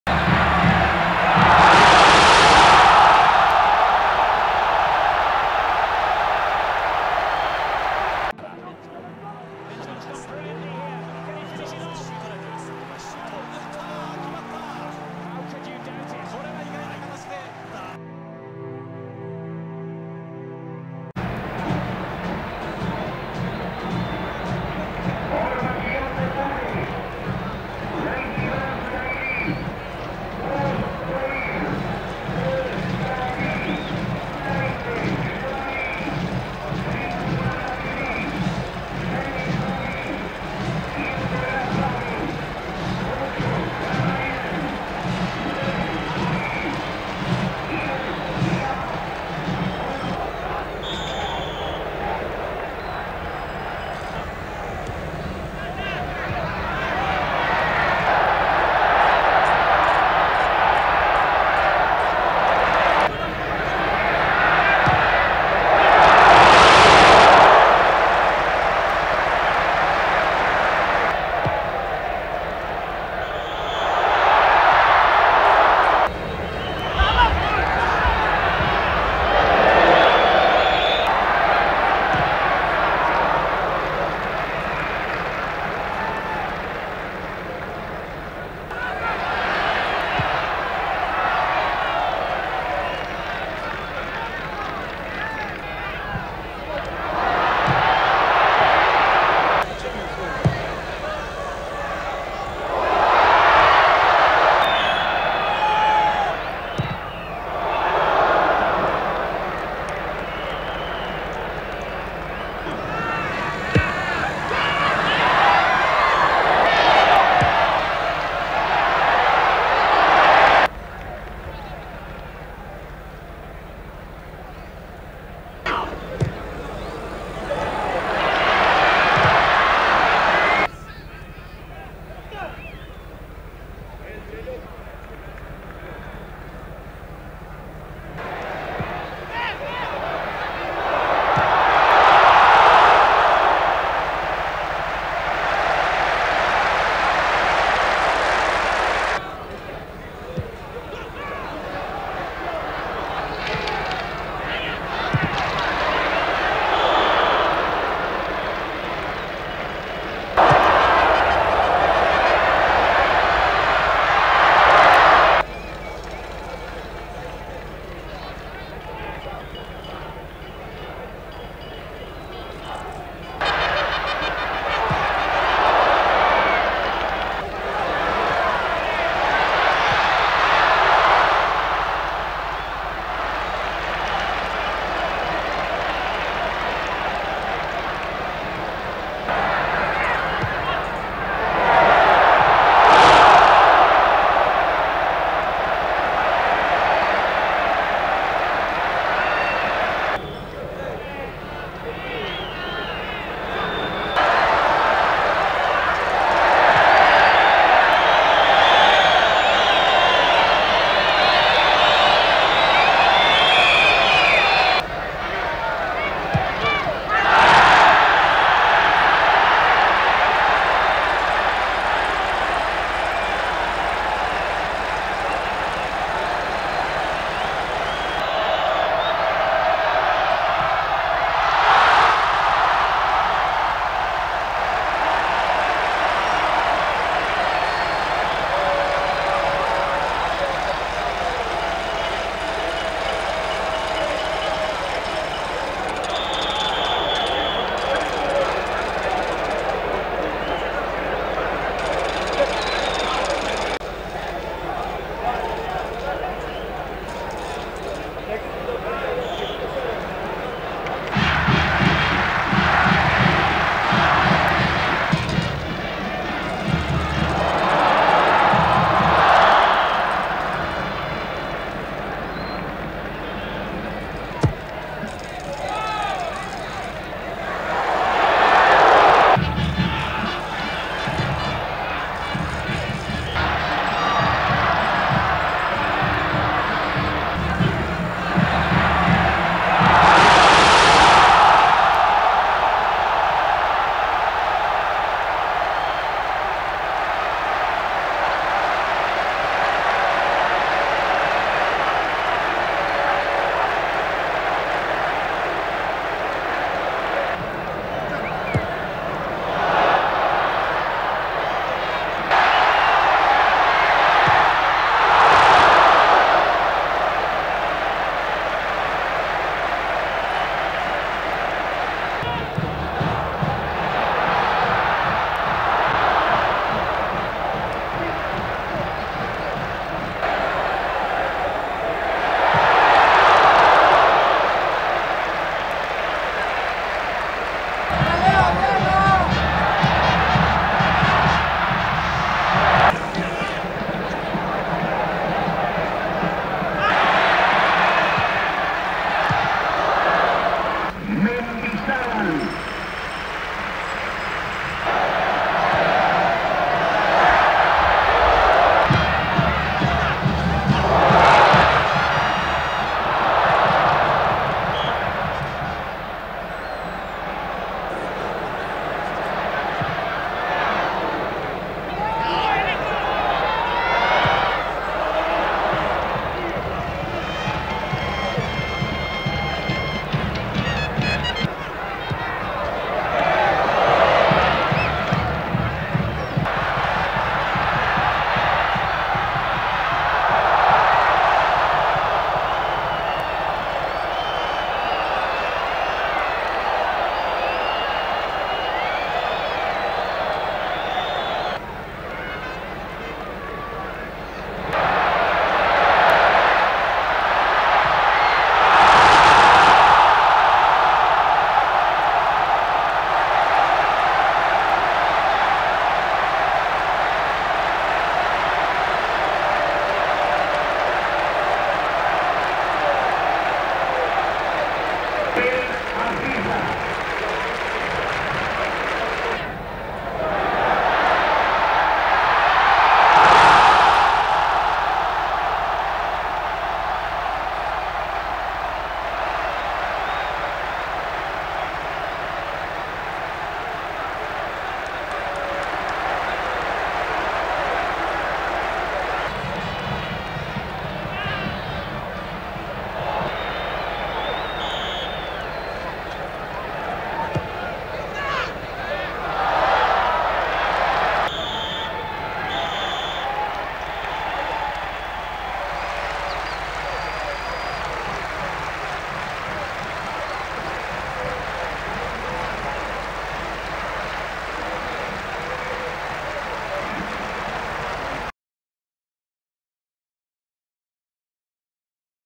Tunisia vs Mexico 3 - 1 Exclusive All Goals World Cup 78 high quality
Tunisia vs Mexico
2 June 1978
16:45 ART
Estadio Gigante de Arroyito, Rosario
Attendance: 17,396
Referee: John Gordon (Scotland)
All Goals

Tunisia
Kaabi 55'
Ghommidh 79'
Dhouieb 87'

Mexico
  Vázquez Ayala 45' (pen.)